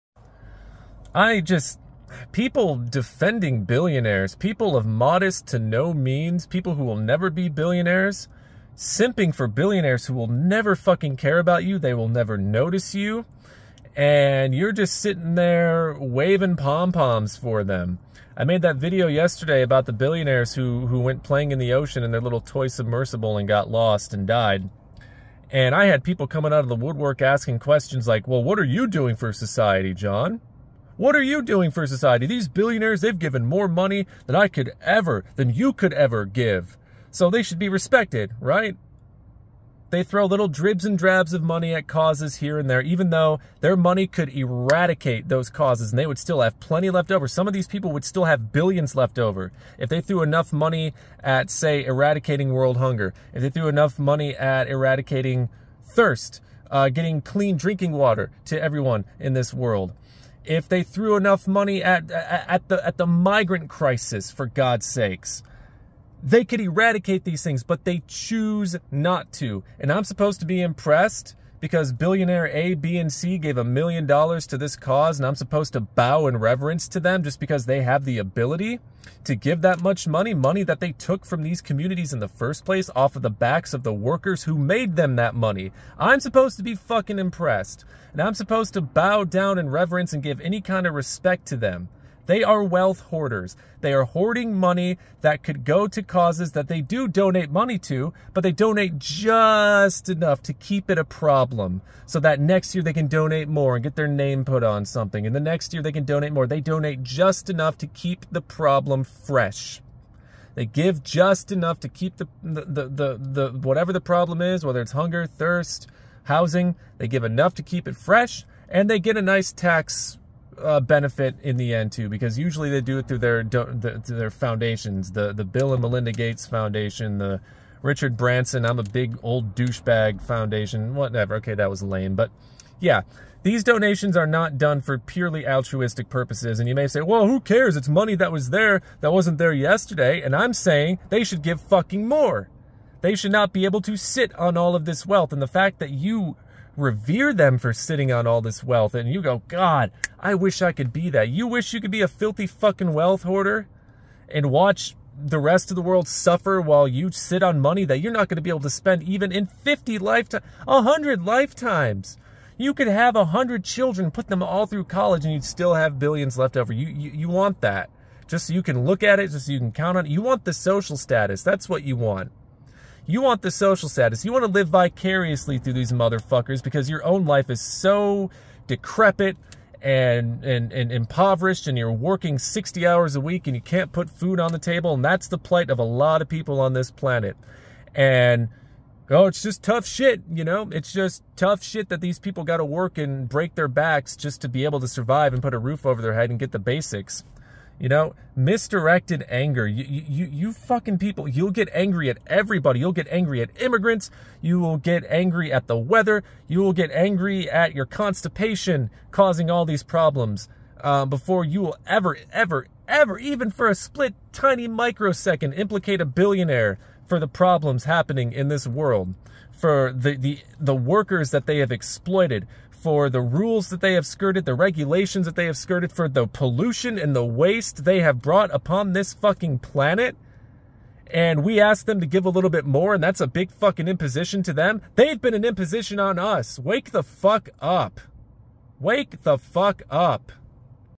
Billionaire worship: more pointless than God worship
My Discord | This is something of a response to some of the comments from my previous video; they're comments I've seen and heard for my entire life with regards to defending billionaires and other wealth hoarders.